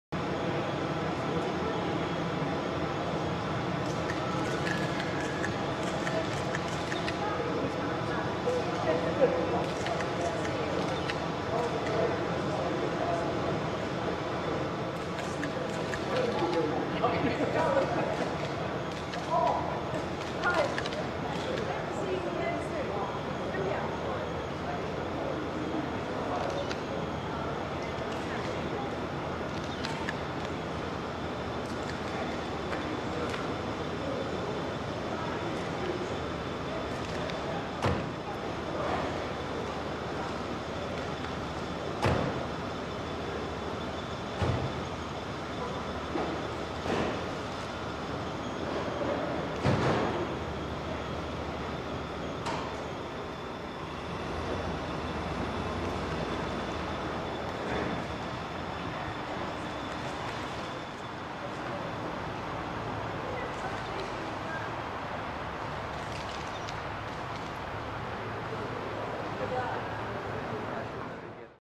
The Queen and her corgis wave off George H. W. Bush in 1989 - Daily Mail
They have been a familiar site at the Queen's heels ever since she was presented with one for her 18th birthday in 1944. But now the Queen - who is known to be inseparable from her beloved Corgis - has decided against introducing any new dogs to Buckingham Palace. She no longer replaces the pets as they die, as she gets older, she is worried about tripping over the small dogs.